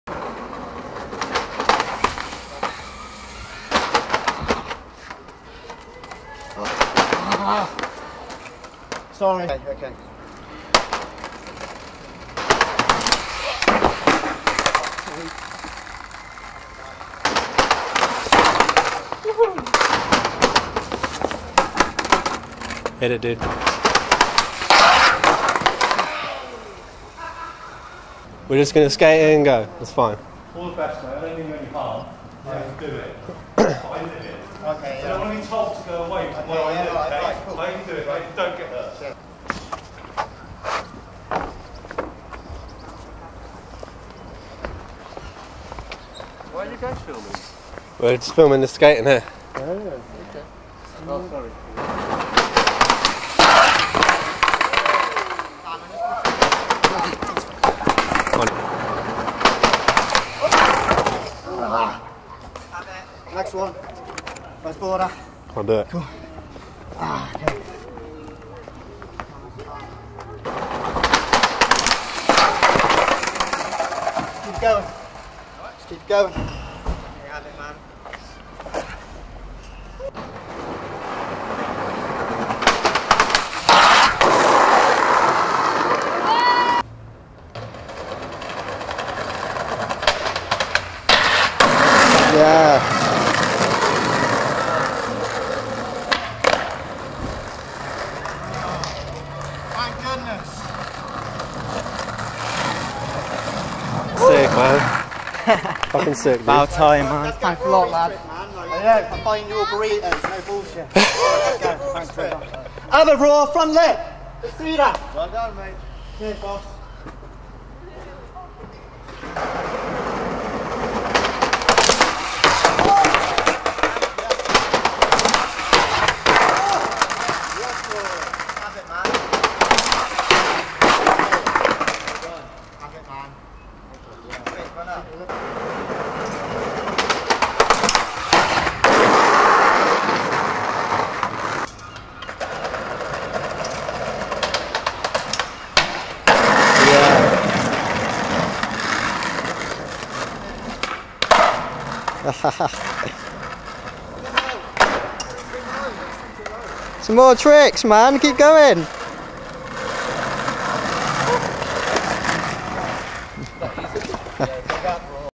Rory Mclean & Turkey Odeon Car Park Rail Sesh, Bath (2016)
Handrail Sesh back in 2016 featuring Rory Mclean and Daniel 'Turkey' Murphy.